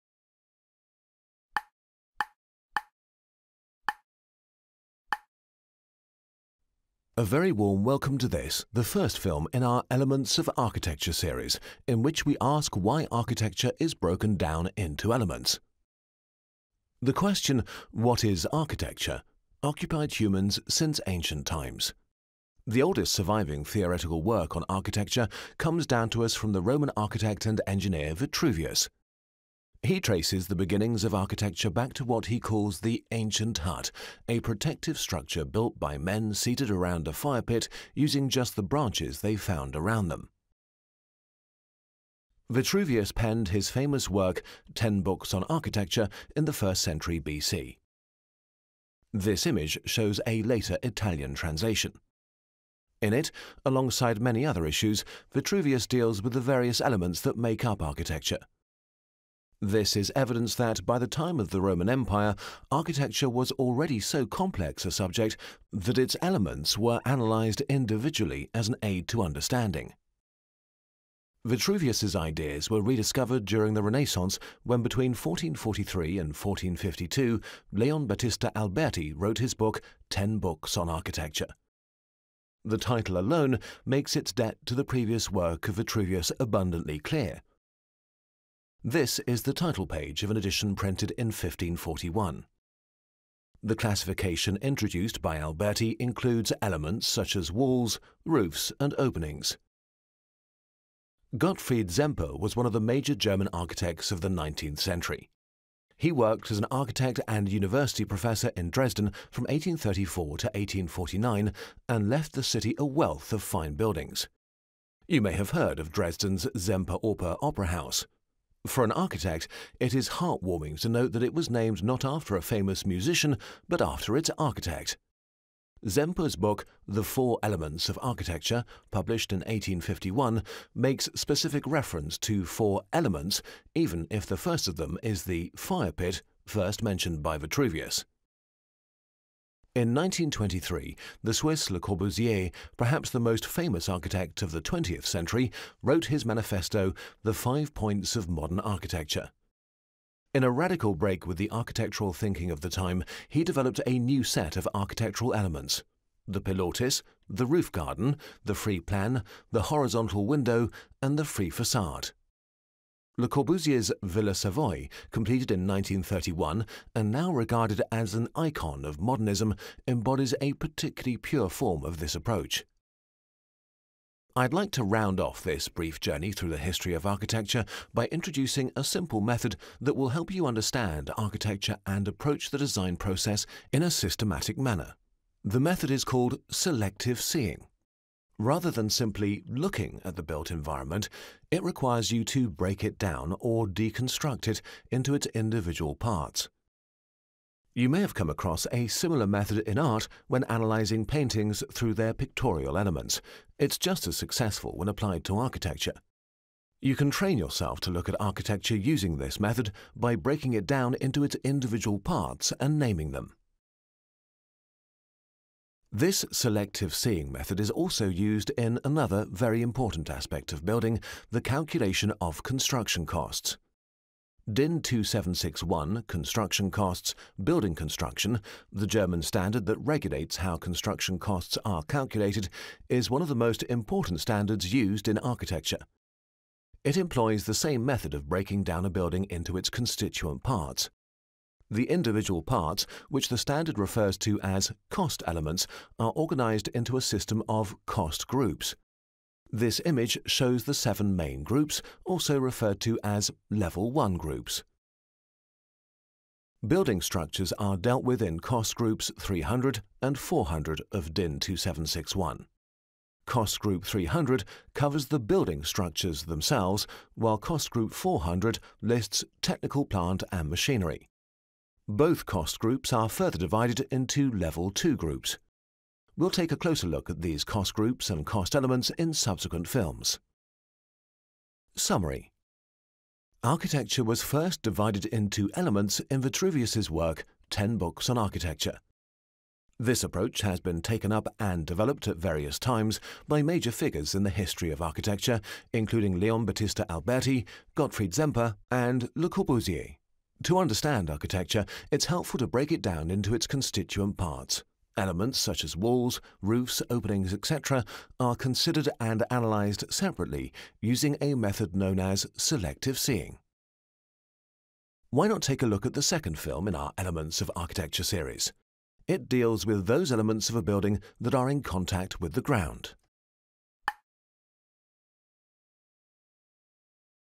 Elements of Architecture | Series 5 – Film 1
Why do you break architecture down into ‘elements’?

Lecture Series for the first year of learning architecture.
Learn with us: 23 Topics and 138 Films on the Fundamentals of Architectural Design and Construction.

TU Dresden CHAIR OF ARCHITECTURAL DESIGN AND CONSTRUCTION
Prof. Ansgar Schulz, Prof. Benedikt Schulz

Design and construction build on each other, from the big picture down to the smallest detail. The architectural space, which the design concept circumscribes in an abstract manner, gradually takes shape in the creation. Likewise, the constructed space only gradually takes on a concrete form until it is finally filled with materials during actual construction.